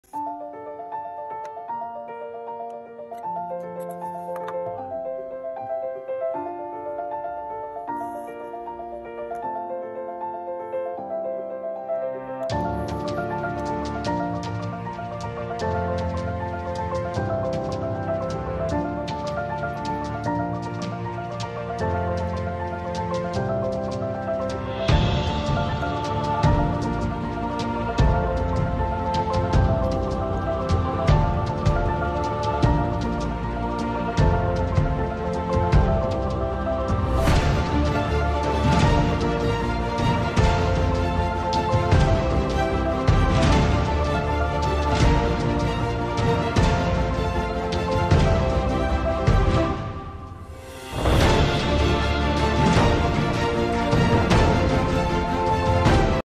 How to fill a Kaweco Sport Fountain Pen
Did you know you can fill the Kaweco Sport pocket fountain pen three different ways? We show you how to fill the new Kaweco Collection Sage fountain pen using an ink cartridge, a Sport piston converter, and an eyedropper (for maximum ink capacity).